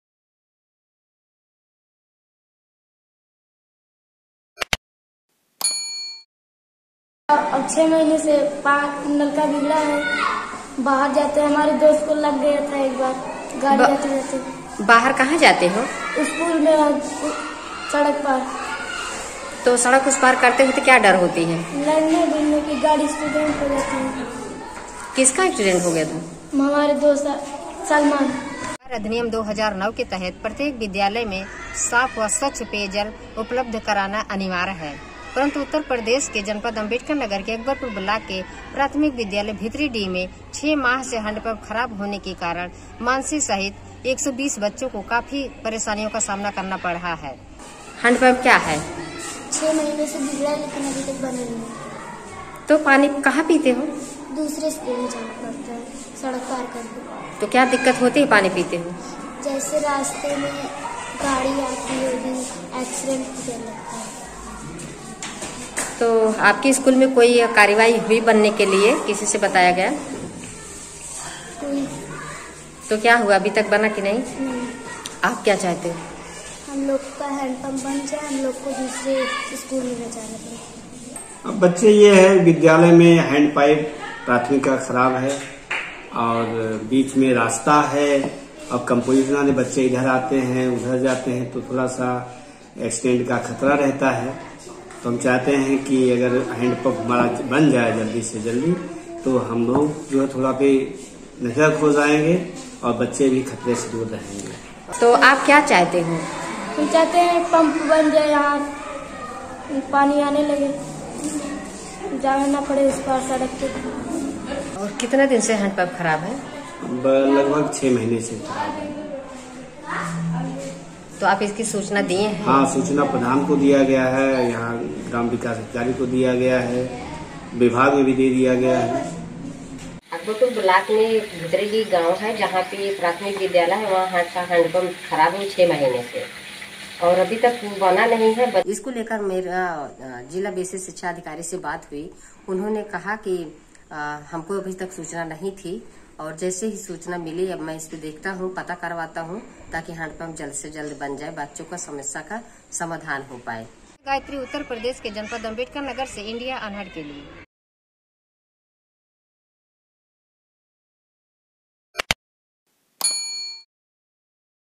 पानी के लिए सड़क पार कर रहे बच्चे की हुई दुर्घटना
उतर प्रदेश के जनपद अम्बेडकरनगर के ब्लाक अकबरपुर के प्राथमिक विद्यालय भितरीडीह मे छ. माह से हैंडपम्प खराब है। जिसमे स्कूल के 120 बच्चो को काफी समस्या का सामना करना पड रहा है। बच्चों को सडक पार कर दूसरे स्कूल में पानी पीने जाना पड रहा है। और इस दौरान एक बच्चे को दुर्घटना के कारण गंभीर चोट भी आयी

शिक्षा का अधिकार अधिनियम 2009 के तहत स्कूल में साफ और स्वच्छ पानी उपलब्ध होना अनिर्वाय है। शिक्षकों द्वारा अधिकारीयों को सूचित किया गया पर अभी तक की कार्यवाही नही  हुई है।

Community Correspondent Gayatri Devi  reports from Ambedkar nagar, Uttar Pradesh  for Video Volunteers.